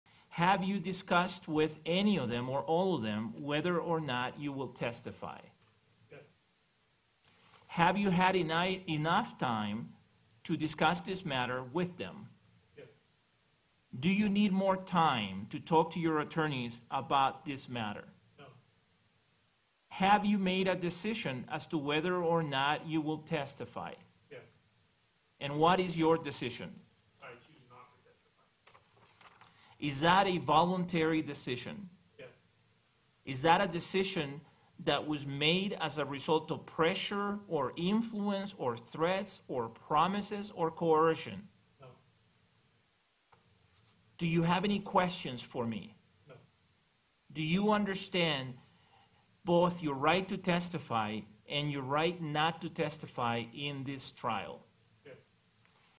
James Holmes speaks in court: I choose not to testify
James Holmes spoke in court for the first time since the start of the Aurora movie theater shooting trial, saying he chooses not to testify.